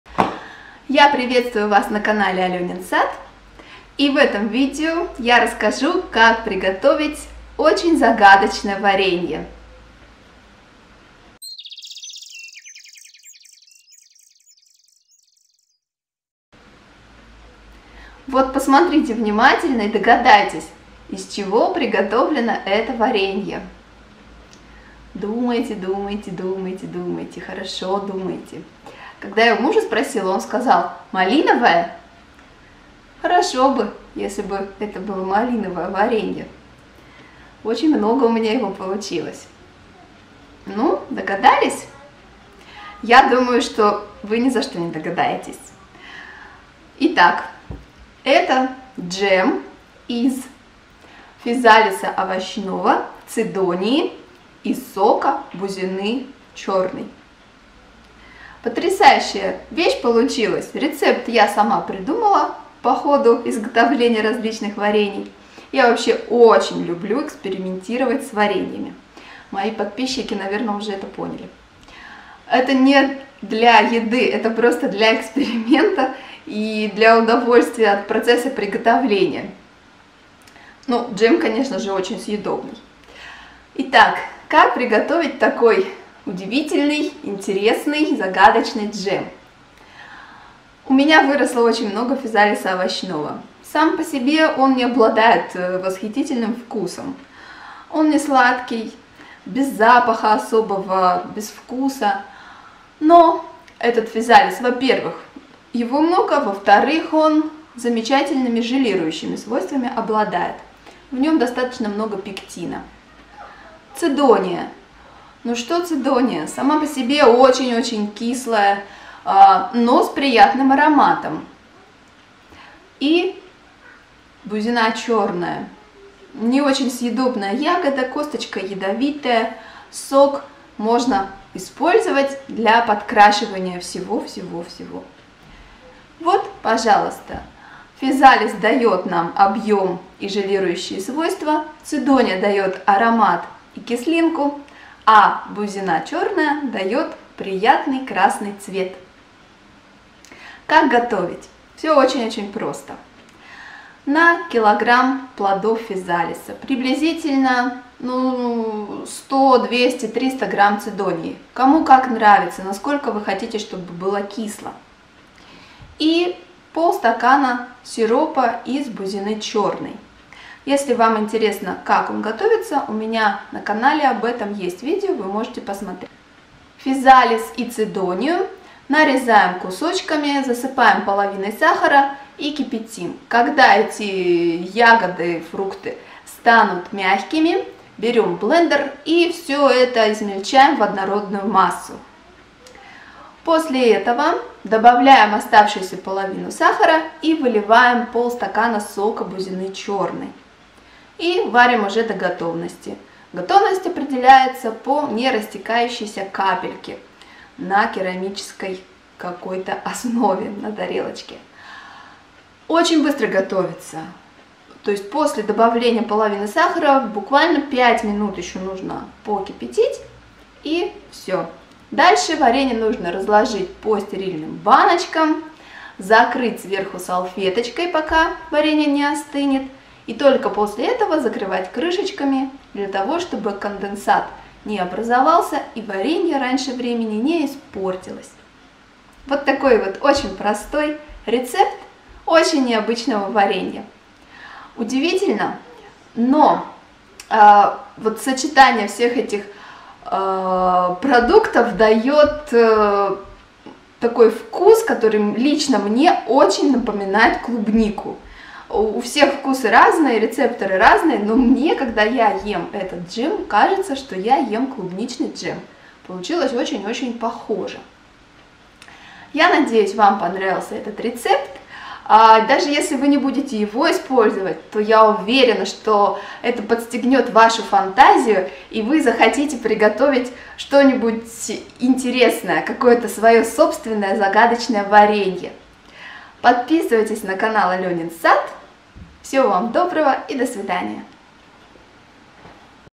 Варенье из физалиса овощного и бузины
В этом видео я расскажу рецепт очень необычного и загадочного варенья! Не у всех будут ингредиенты для него, но вдруг так сложится в жизни и вы его тоже приготовите???
на Facebook: Алена Волкова
в Twitter: @AleninSad
на Instagramm: @alenas.garden
Буду рада новым друзьям!!!